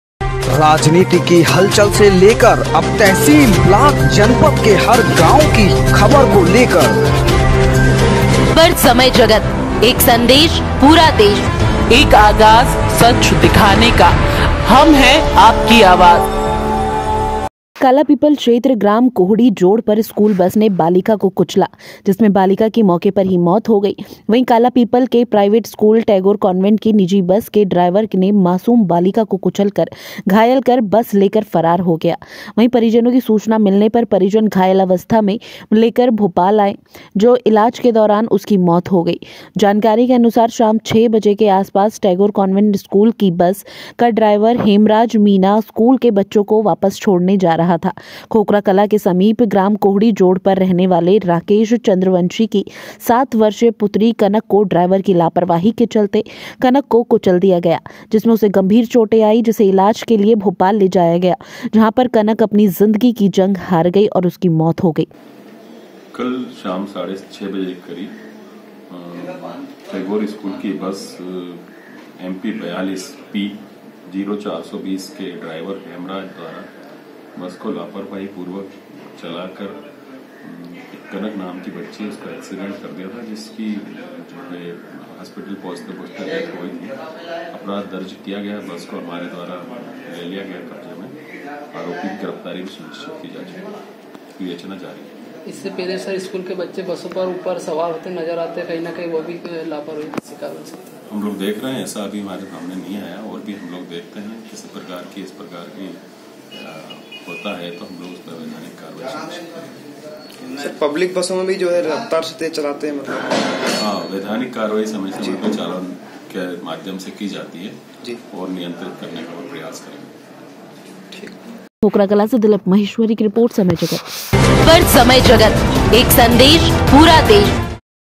खोकराकलॉ- कालिपीपल क्षेत्र ग्राम कोहड़ी जोड़ पर स्कुल बस ने बालीका को कुचला
दिलीप महेश्वरी की रीपोर्ट